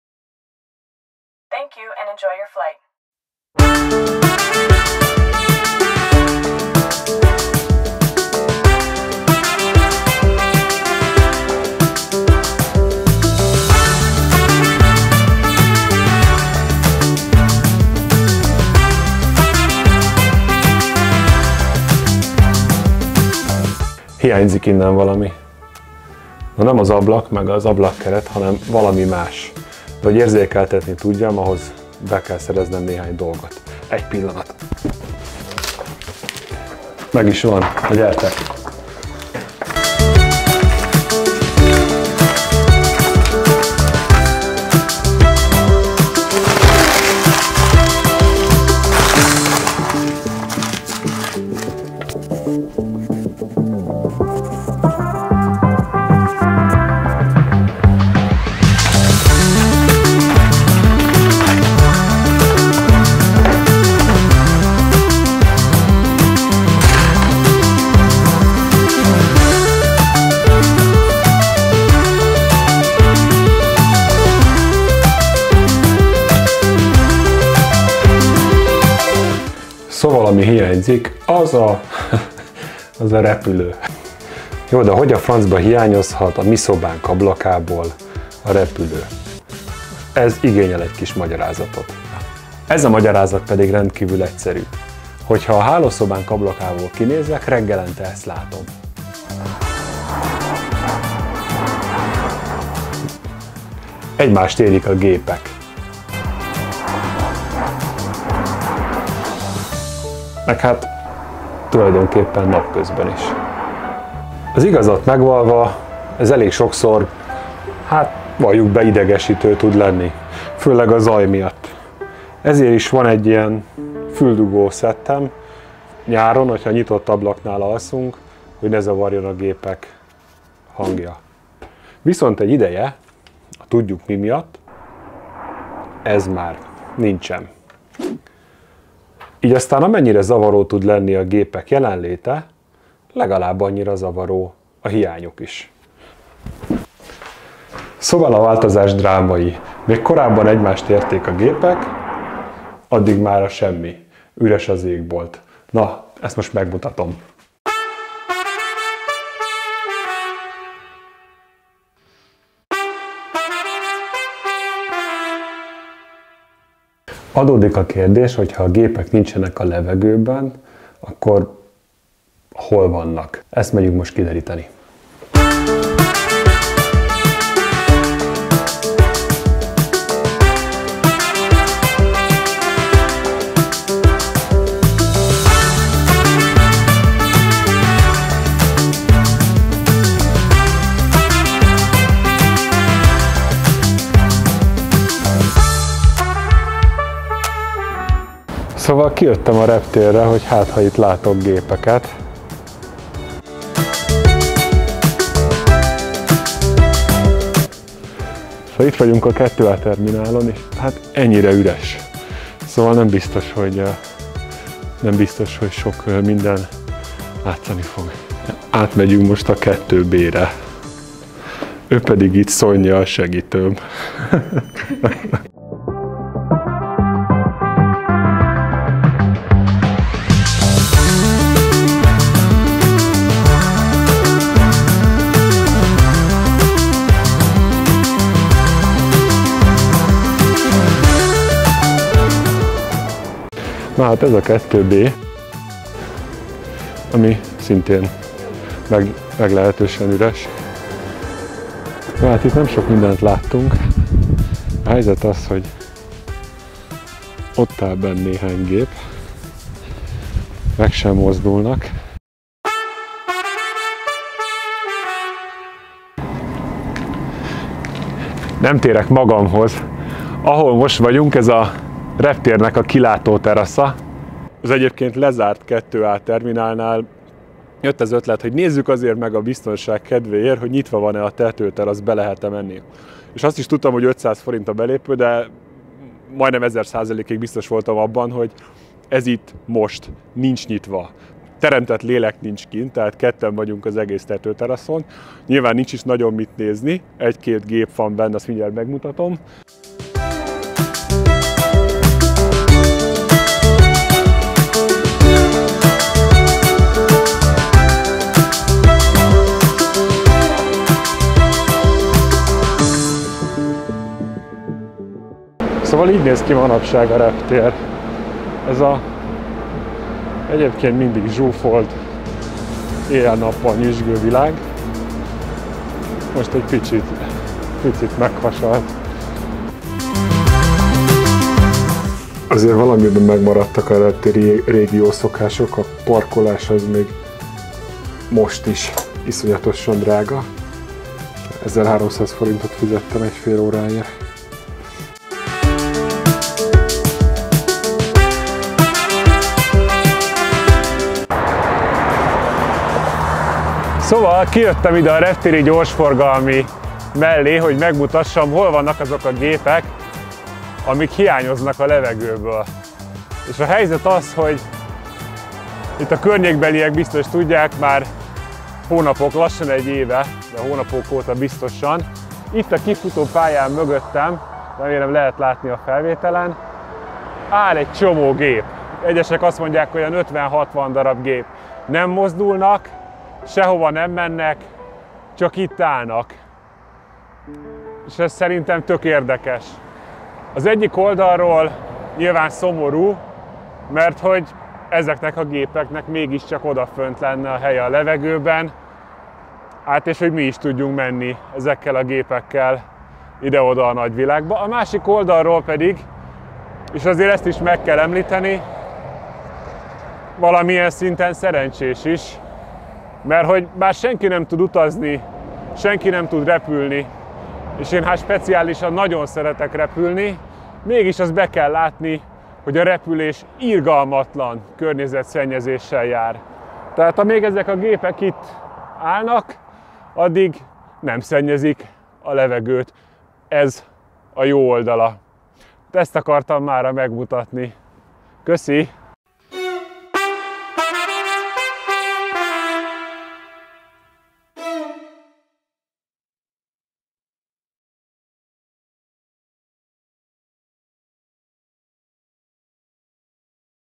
Hova rejtették a repülőket a levegőből?
Hova rejtették a repülőket a levegőből? Ennek járunk utána Ferihegyen, akarom mondani a Liszt Ferenc reptéren, ahol a parkolás a régi, minden más nem. Budapest Airport, ahogy ritkán látod. Valamint szó lesz még az ablakomról. :)

Szonja 03:42
03:53
03:58
04:22
05:08